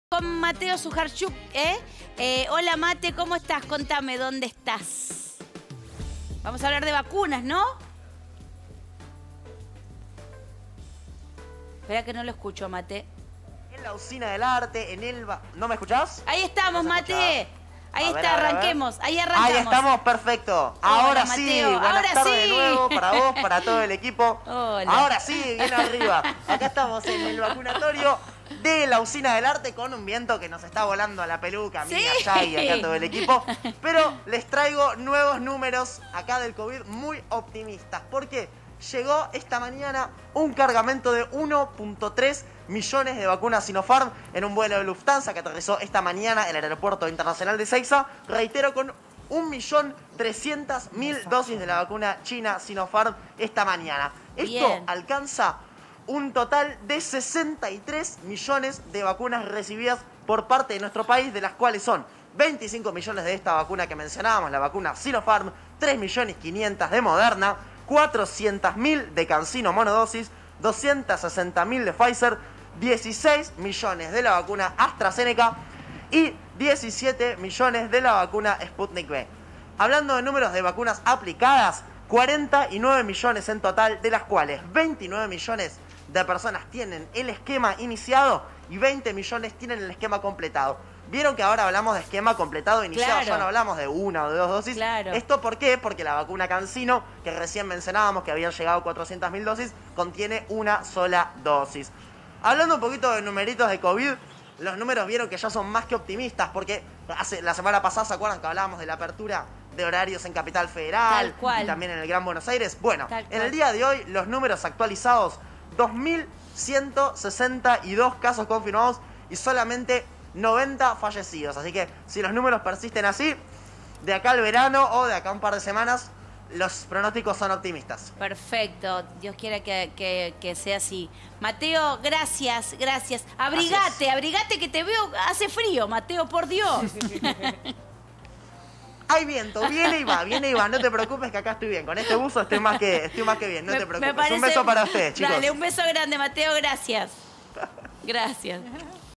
Vacunación en Argentina: Se han aplicado más de 63 mil millones dosis
Argentina recibió 36 millones de dosis desde inicio de la pandemia

La Argentina ha recibido 63 mil millones de dosis de vacunas contra la covid 19 desde inicio de la pandemia.

Con la conducción de Maju Lozano. De lunes a viernes a las 17.30hs por El Nueve.